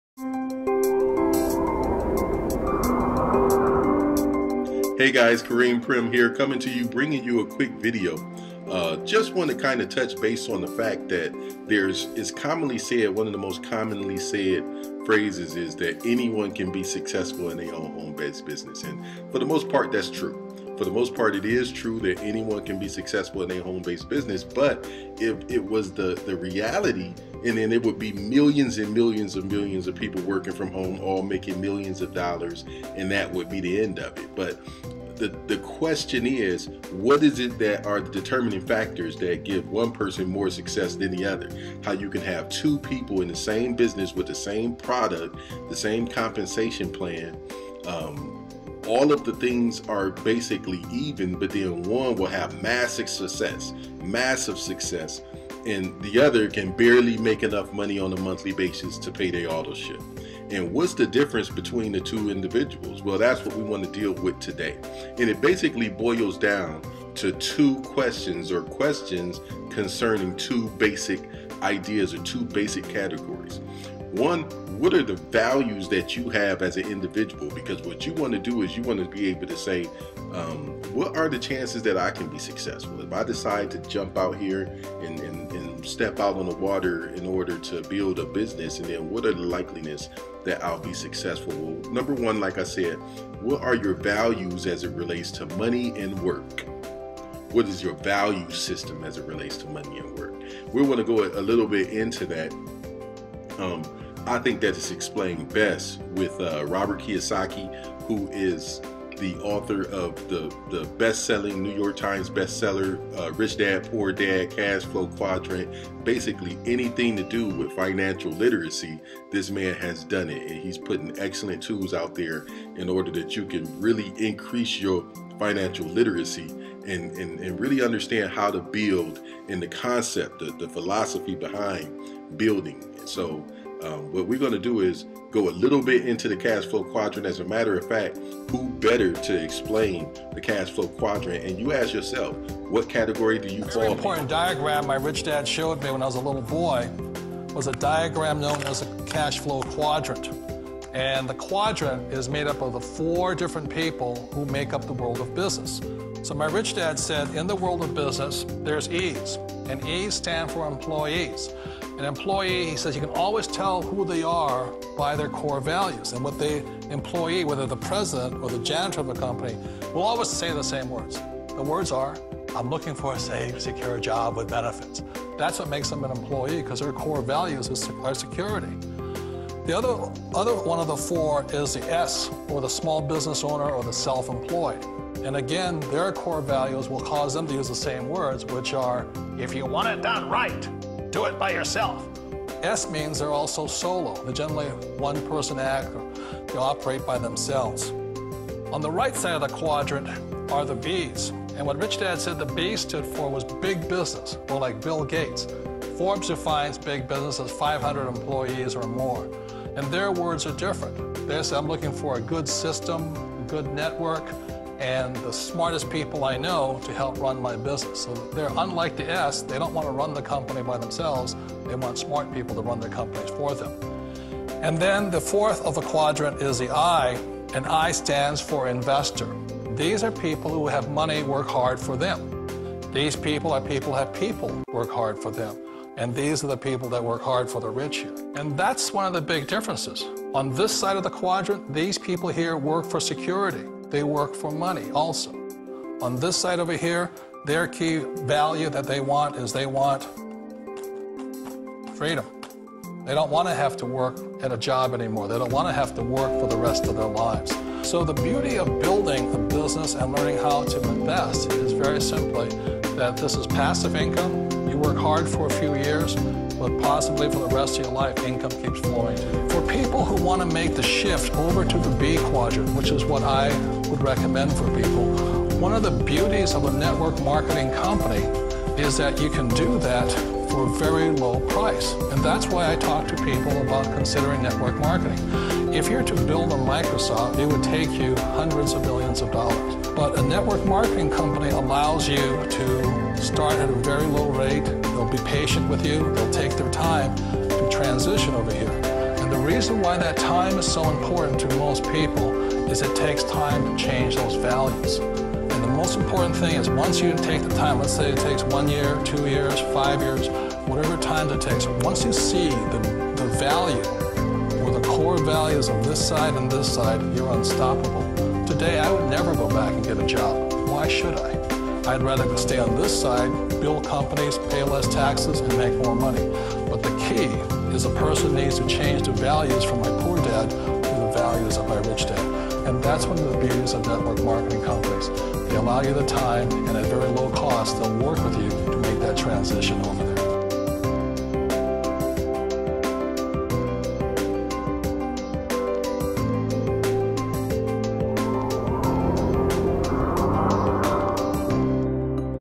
Do You Have What It Takes To Work From Home? - Kariem Primm
Find out if you have a greater or lesser likeliness of having success building a business from home. We can help you start and or grow your home-based business.